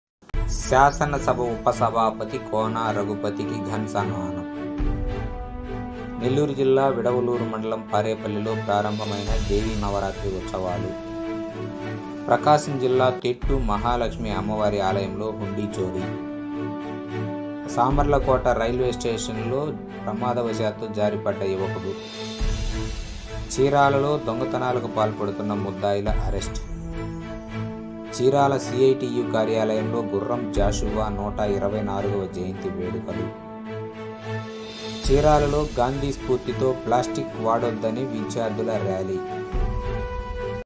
29 09 19 Hedlines
1. శాసనసభ ఉపసభాపతి కోన రఘుపతి కి ఘన సన్మానం
2. నెల్లూరు జిల్లా విడవలూరు మండలం పారేపల్లి లో ప్రారంభమైన దేవీ నవరాత్రి ఉత్సవాలు
 3. ప్రకాశం జిల్లా తిట్టు మహాలక్ష్మి అమ్మవారి ఆలయంలో హుండీ చోరీ
 4. తూర్పుగోదావరి జిల్లా సామర్లకోట రైల్వే స్టేషన్లో విశాఖ ఎక్స్ప్రెస్ రైలు కింద ప్రమాదవశాత్తు జారి పడ్డ యువకుడు
 5. చీరాలలో దొంగతనాలకు పాల్పడుతున్న ముద్దాయిల అరెస్ట్
 6. చీరాలలో సిఐటియు కార్యాలయంలో గుర్రం జాషువా 124వ జయంతి వేడుకలు
 7. చీరాలలో ప్లాస్టిక్ రహిత దేశం నిర్మాణం కోసం విద్యార్థుల ర్యాలీ
 8. చీరాల వాడరేవులో డివైఎఫ్ఐ ఆధ్వర్యంలో భగత్ సింగ్ జయంతి